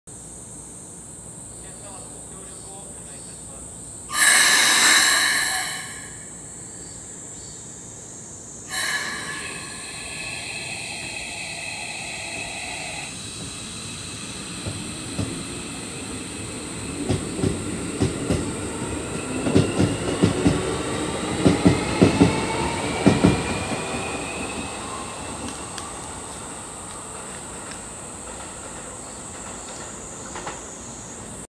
【東芝PMSM】京急 1000形1367編成 エアポート急行 神奈川新町発車
京急で唯一の東芝PMSMを採用している1367編成。

[SNS]